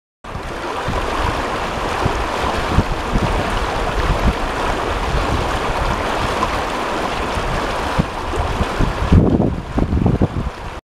川の流れの観察技術 To observe the flow of the river
注：
くりかえし
　超音波と
　流体の変化（流れ、渦、波・・）を
　観察して

1年ぐらい経過してくると

　とらえたいと研究しています

音響流
一般概念

音響流は、
　または
　音場内の